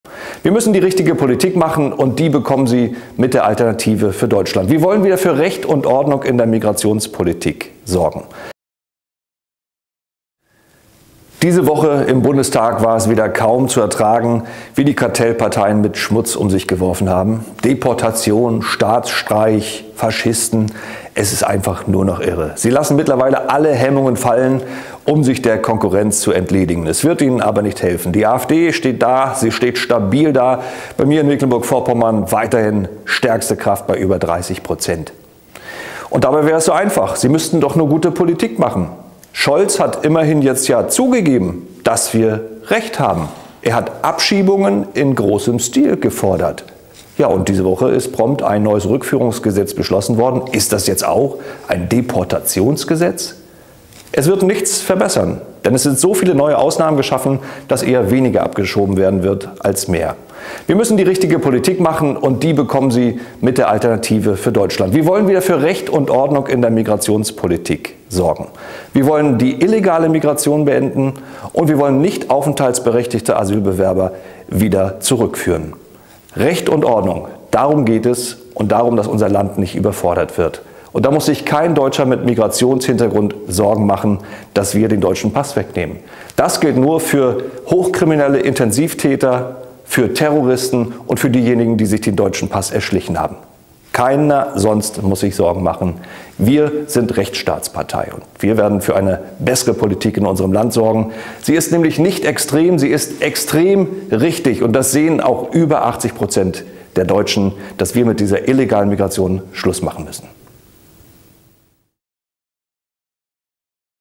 Die AfD steht für Recht & Ordnung in der Migrationspolitik! | Leif-Erik Holm, MdB
Medien und Kartellparteien überbieten sich derzeit in Diffamierungen und Verleumdungen – weil ihnen die AfD zu stark geworden ist. Doch die Kampagne verfängt nicht!Die Bürger wollen endlich einen Neustart, zuallererst in der Migrationspolitik. Mit uns gibt es keine weitere Aufweichung des Staatsangehörigkeitsrechts. Wir machen der illegalen Einwanderung ein Ende. Und nur mit der AfD gibt es eine echte Abschiebeoffensive.